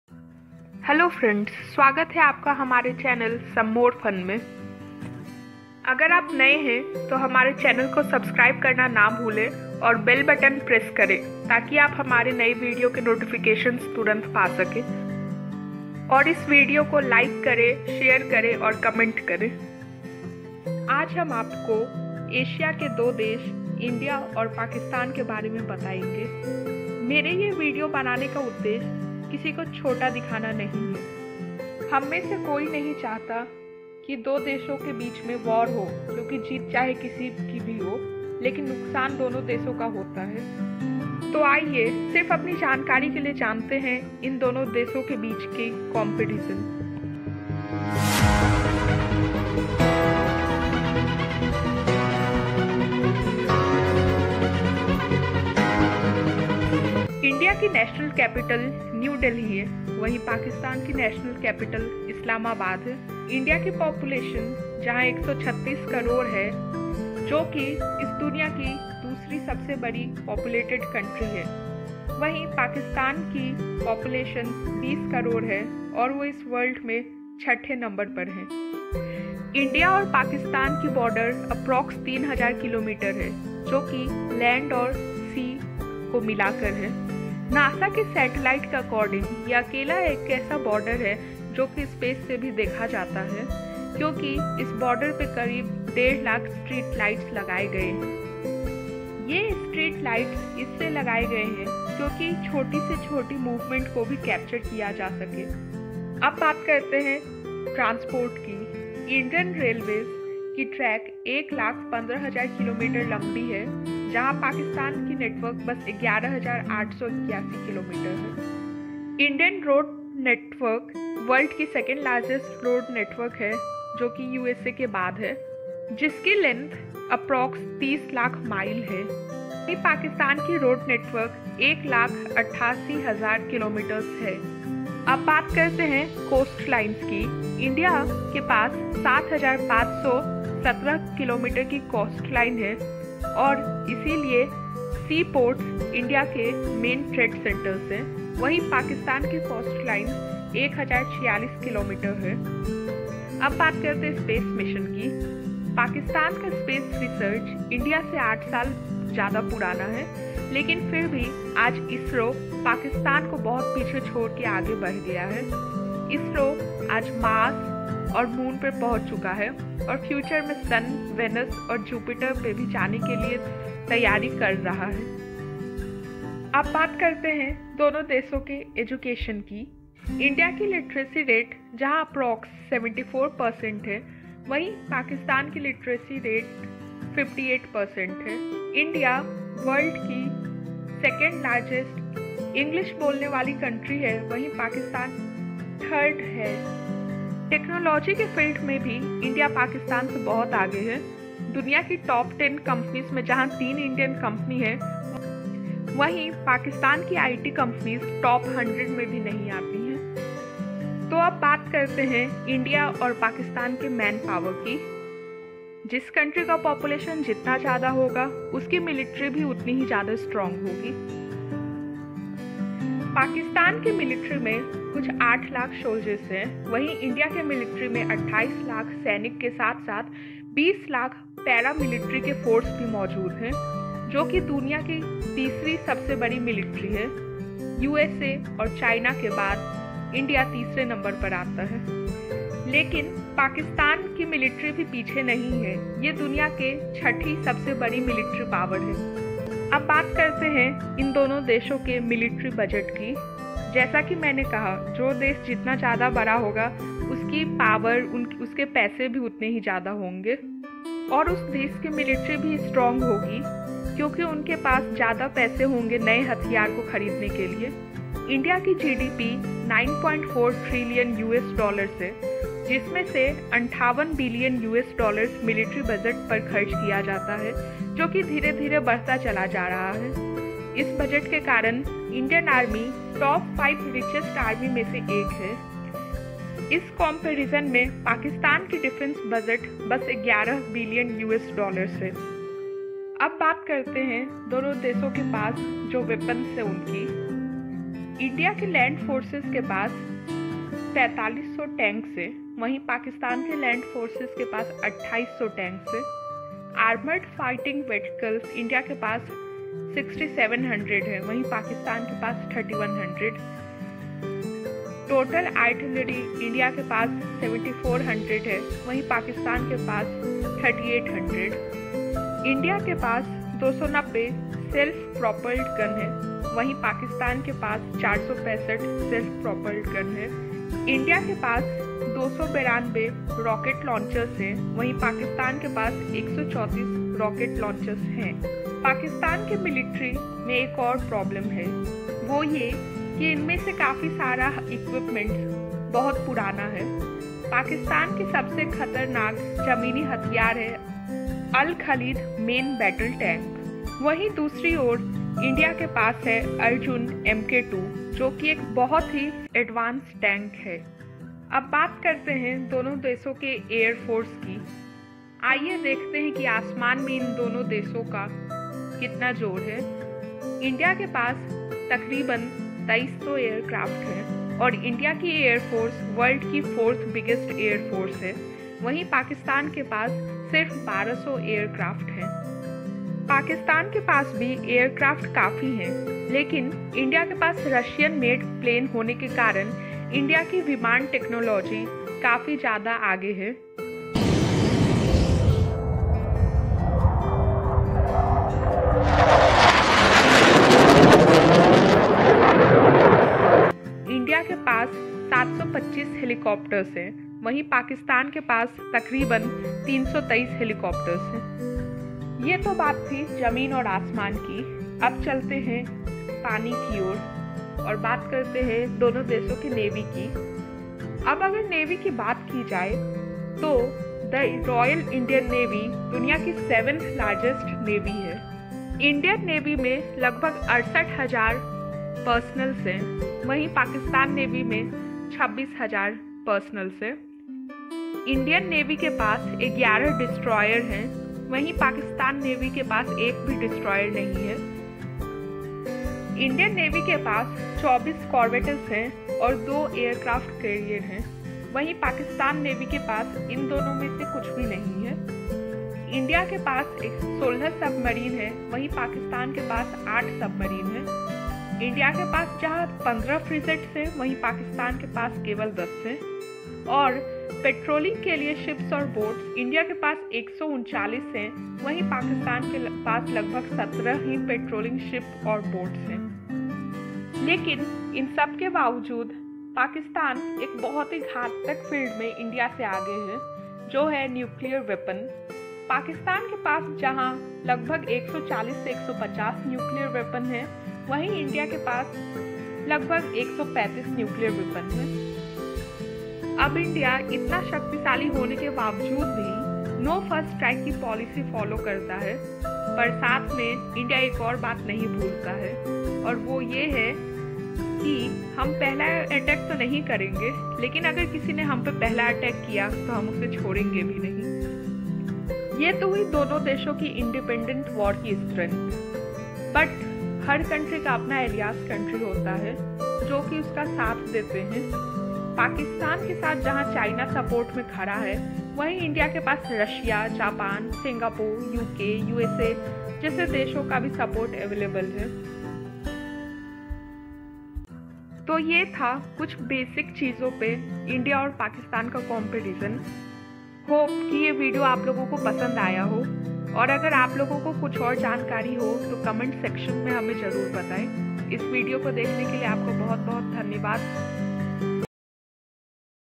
India vs Pakistan || Who is Powerful? || Brief Details || Comparison
In This Vidoe We Are Going To Tell About The Brief Details Of India And Pakistan. We Made This Video Only To Know The Basic Details. One Can Know About The Education, Technology, Transport, Manpower, Weapons, Army, Airforce, Navy Of Two Countries.